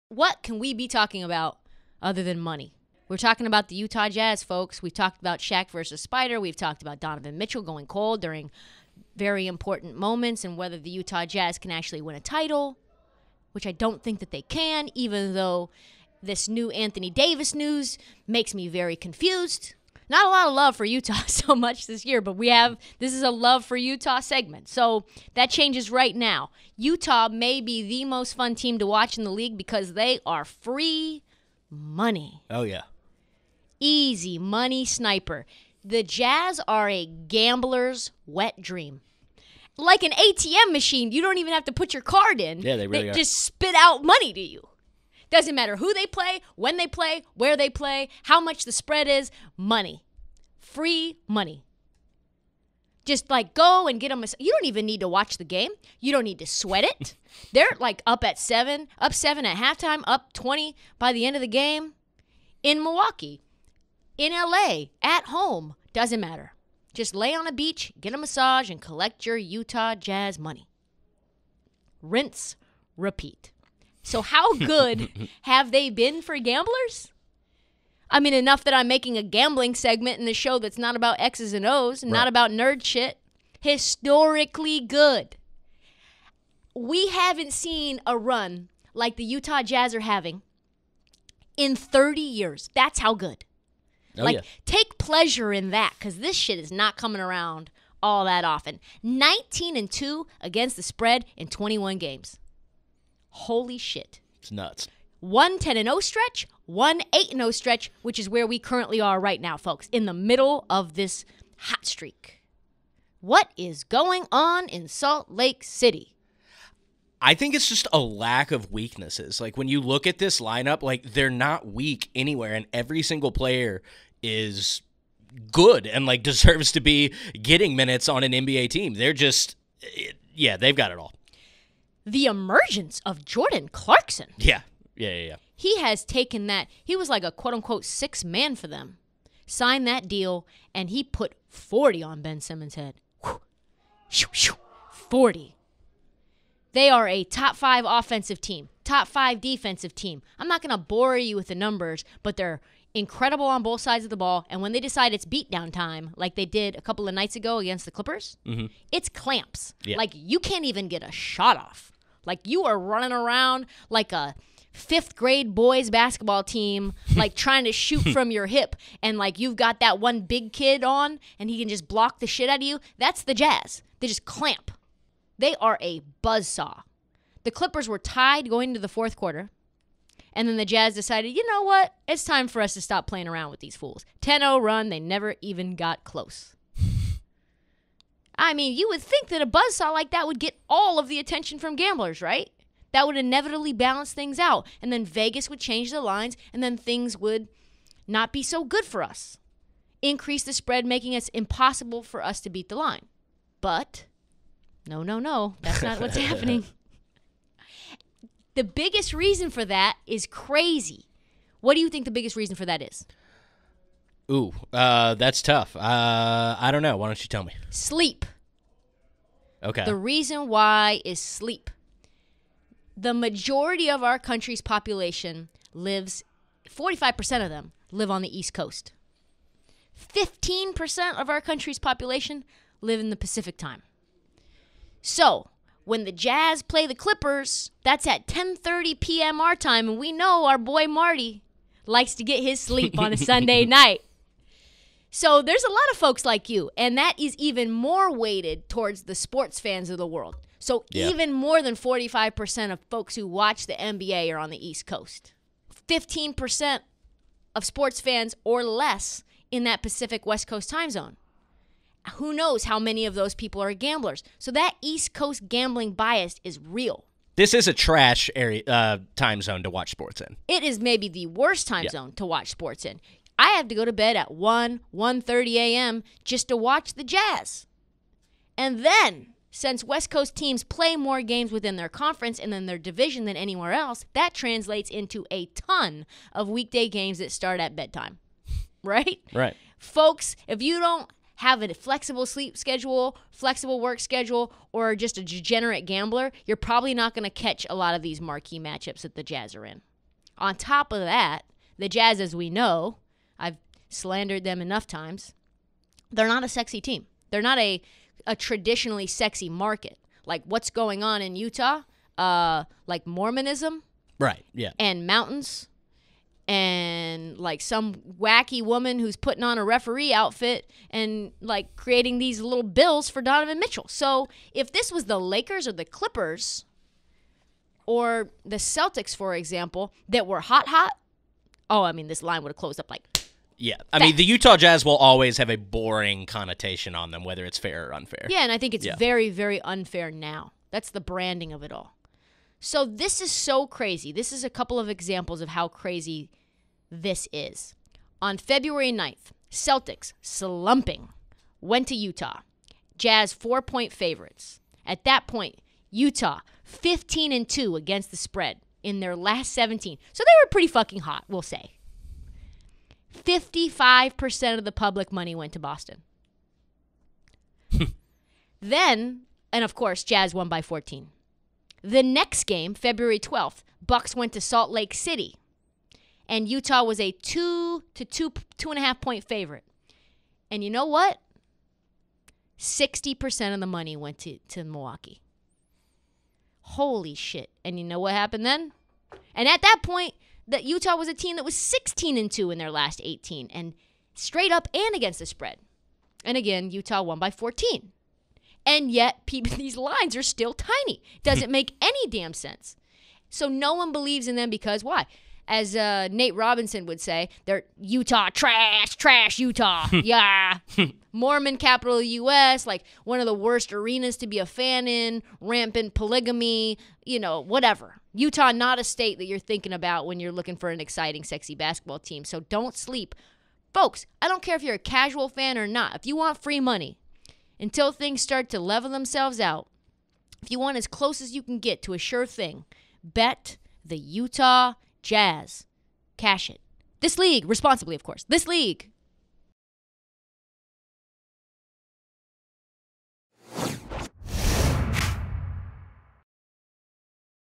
The Utah Jazz Are On A Historic NBA Run For Gambling
Trysta details exactly how crazy Utah's latest run has been.